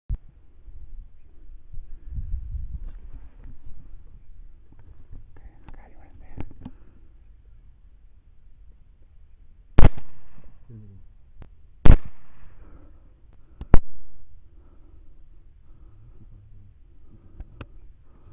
Nilgai Hunt w Thermal in TEXAS
Ross shot a nilgai in the trees with a Pulsar XP50